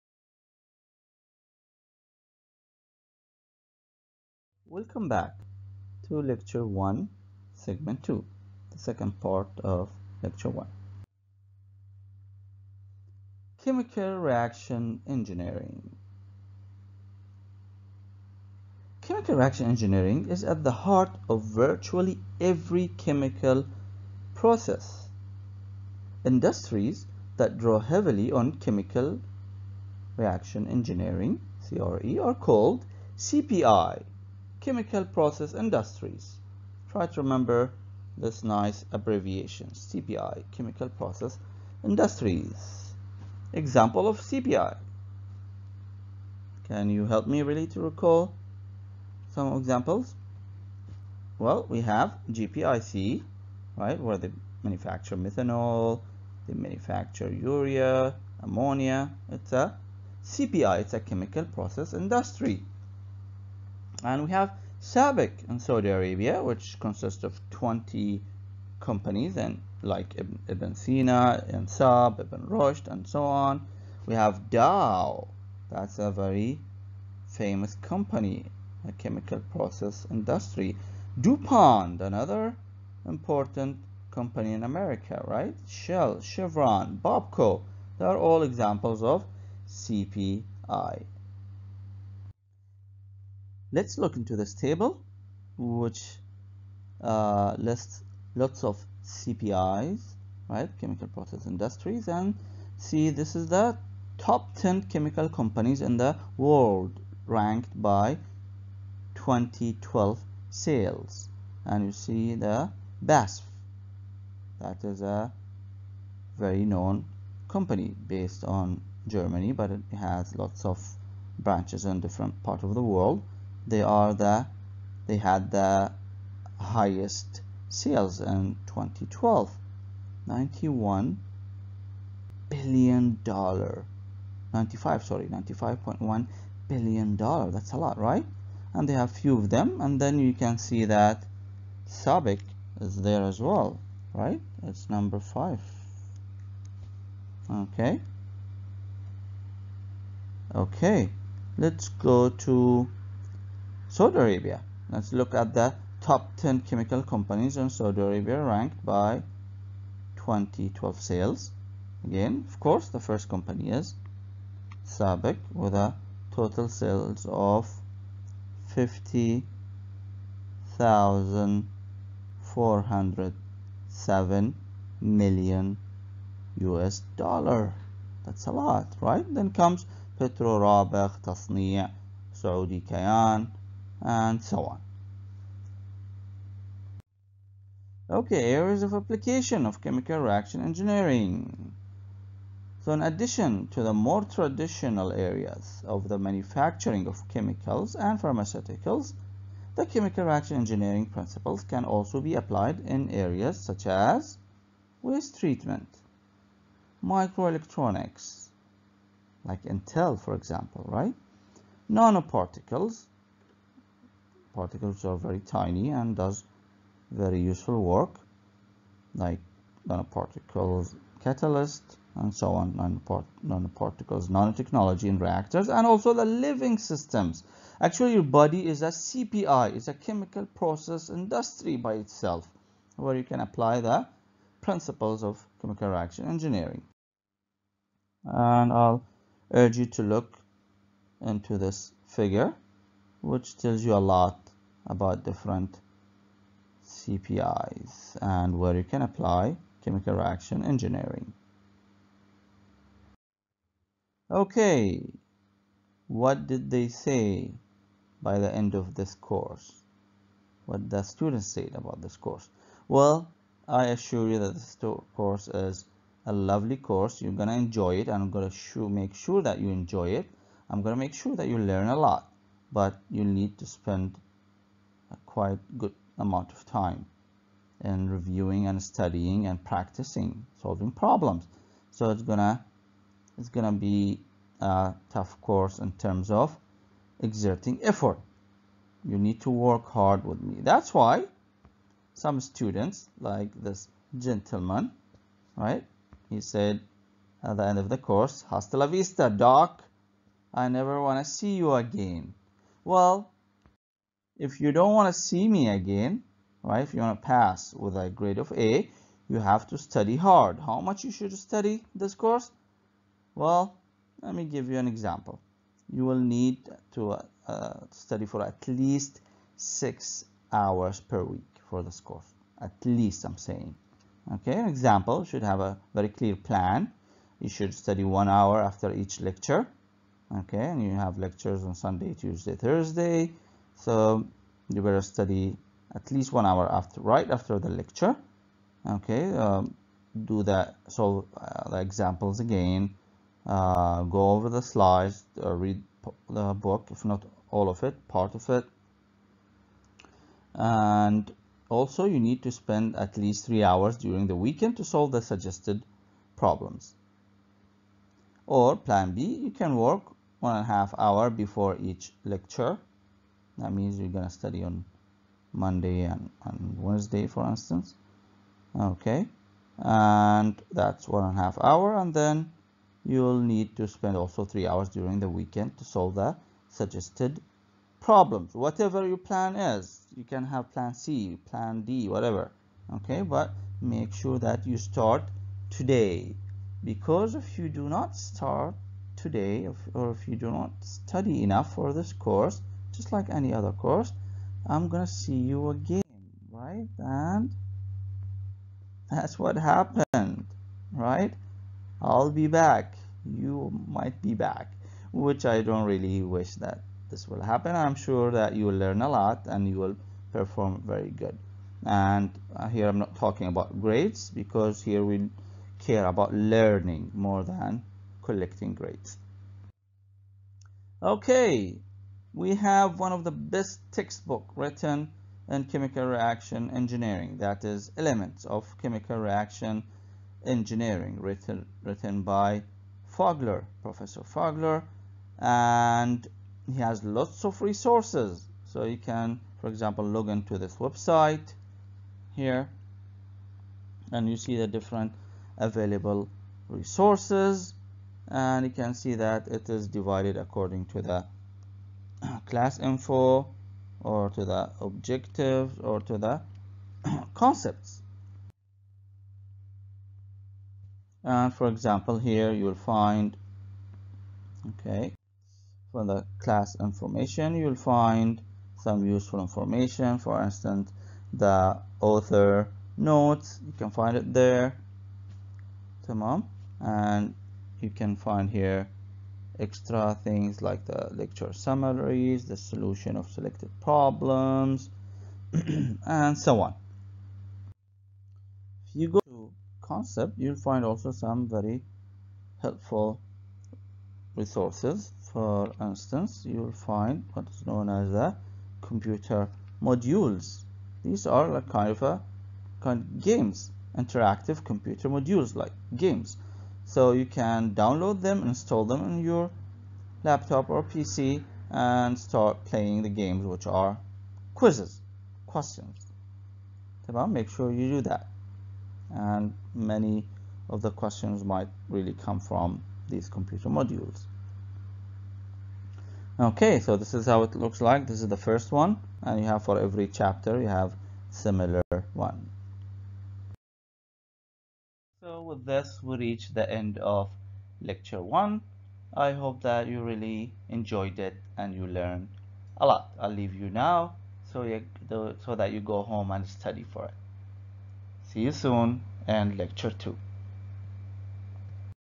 Lecture 1 - Seg 2, Chapter 1, Introduction to Chemical Reaction Engineering (CRE)
This lecture is part of “Chemical Reactor Design” course and it gives a brief introduction to Chemical Reaction Engineering (CRE) and how to study for the course as explained in chapter 1 “Mole Balances” of the textbook “Elements of Chemical Reaction Engineering”, 4th ed., by H. Scott Fogler.